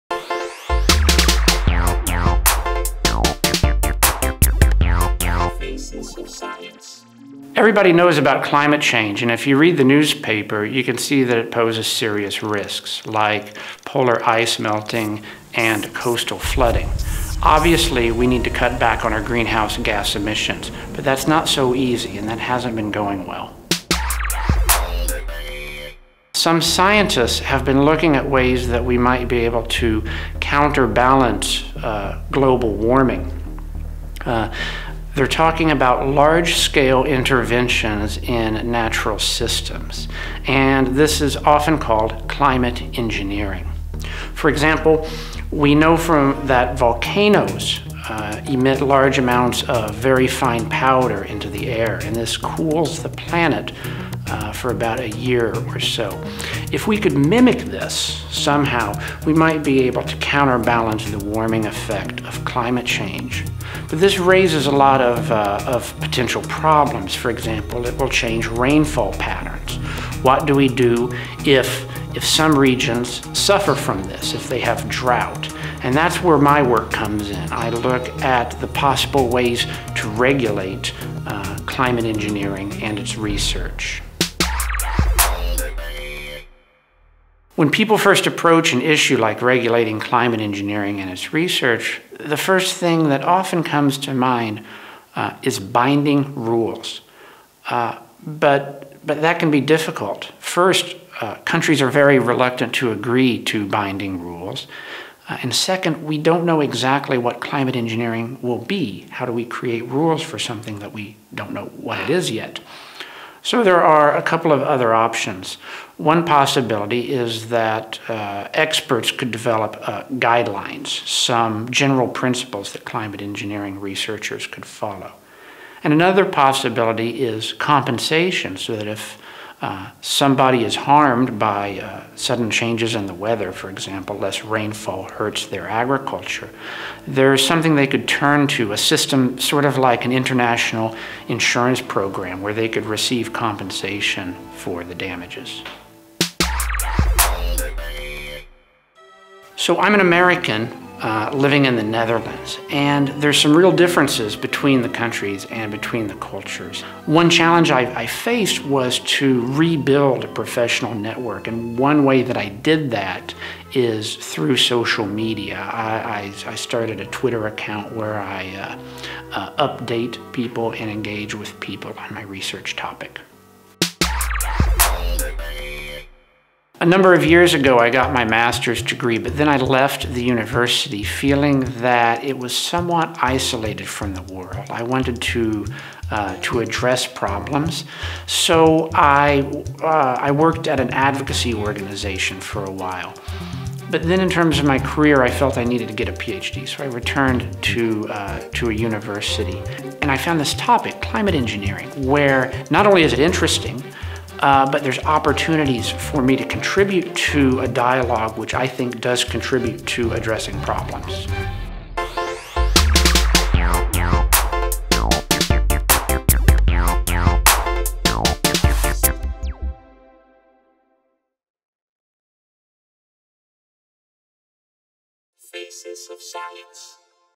Regulating climate engineering
Everybody knows about climate change and if you read the newspapers you can see it poses serious risks. Some scientists have been looking at ways to counterbalance global warming. They talk about large scale interventions in the natural systems: climate engineering. But this poses potential problems like changing rainfall patterns. Jesse Reynolds looks at possible ways to regulate climate engineering and its research.

Gefinancierd door de wetenschappelijke auteurs van Elsevier Science, ondersteund door Lira Auteursfonds Reprorecht

Met beelden van: Jesse Reynolds

Gemaakt door: Moira van Dijk 2014

Meer Faces of Science op facesofscience.nl